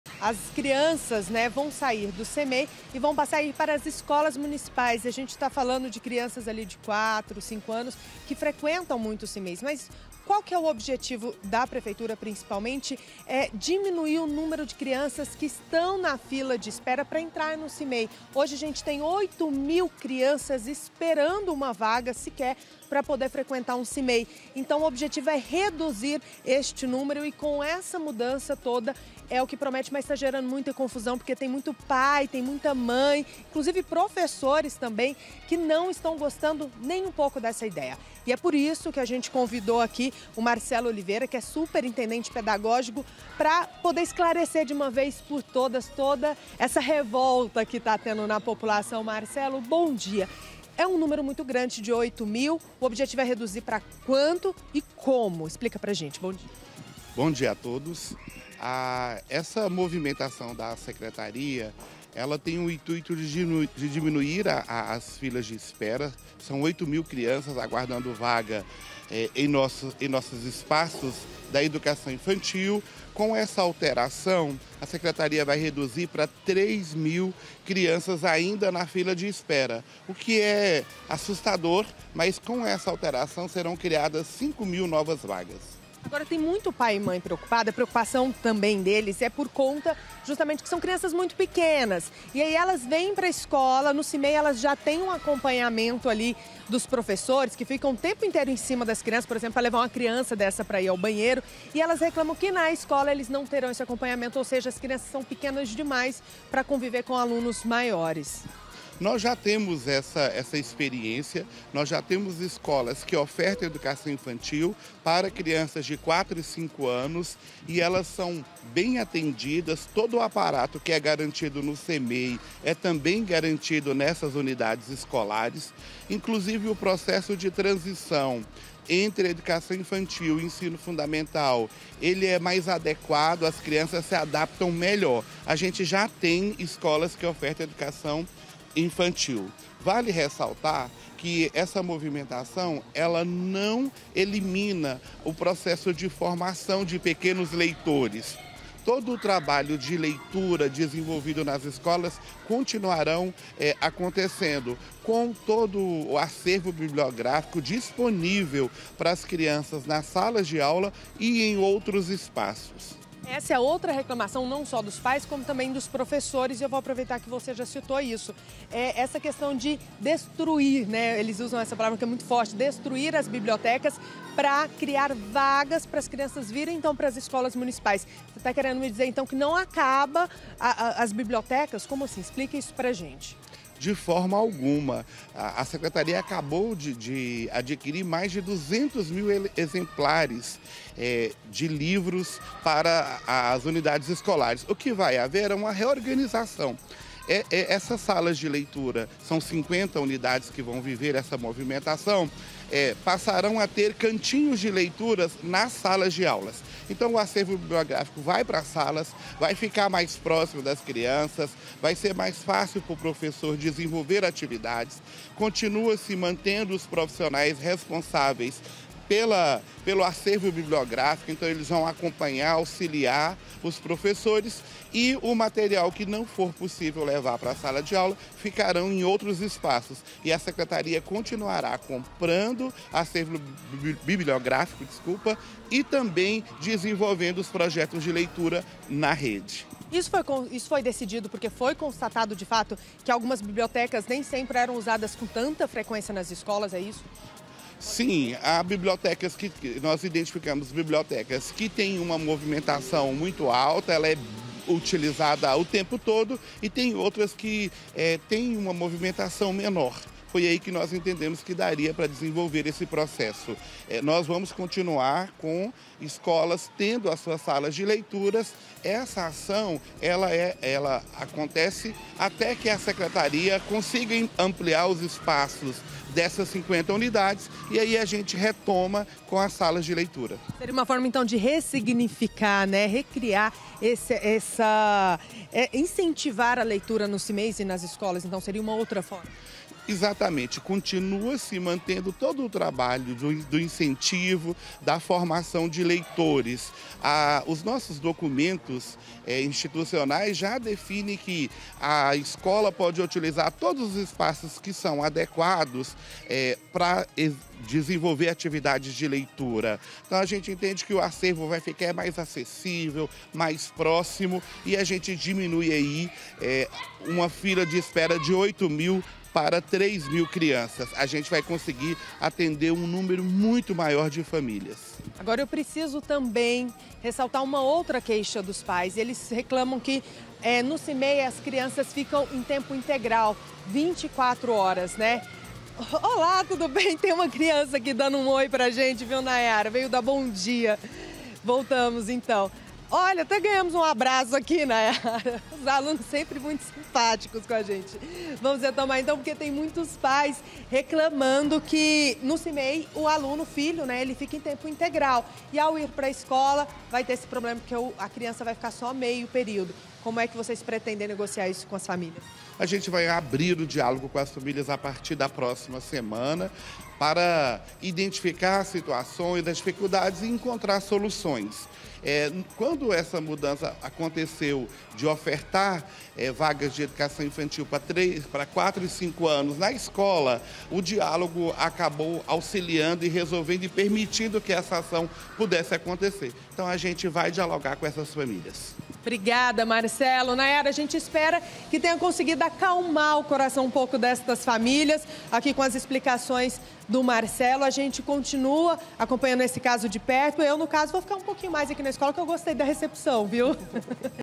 MUDANÇAS NOS CMEIS: MAIS VAGAS PARA ALUNOS ATÉ 3 ANOS
VAI TER MUDANÇAS NOS CMEIS DA CAPITAL.AS TURMAS VÃO SER AMPLIADAS POR CONTA DE UM REMANEJAMENTO.